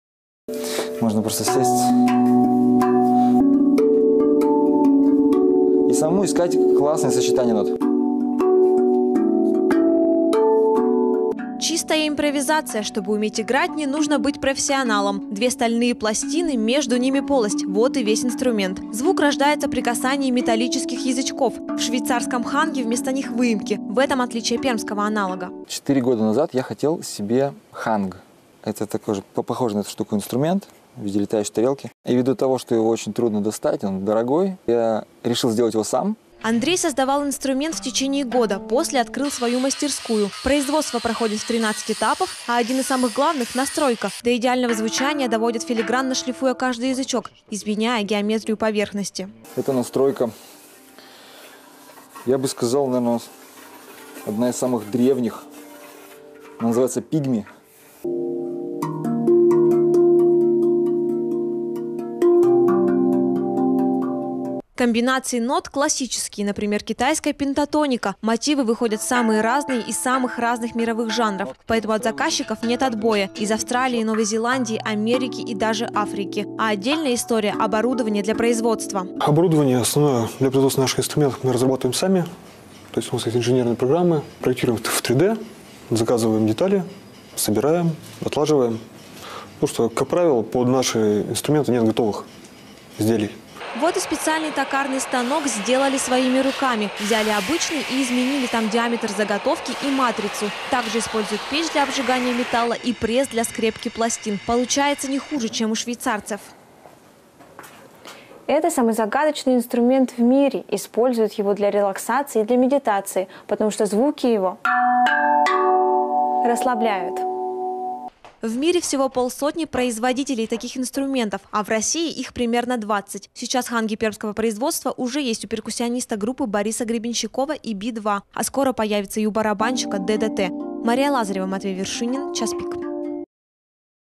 Creation of RAV Drum
Some words about history, manufacturing, and tuning of RAV Drum from Perm TV channel. This video is in Russian, but we promise to add English subtitles soon!
Some years ago Andrey Remyannikov invented a whole new kind of instrument - RAV Drum. And people loved RAV. It was the first steel tongue drum to possess a total of four partials per tongue.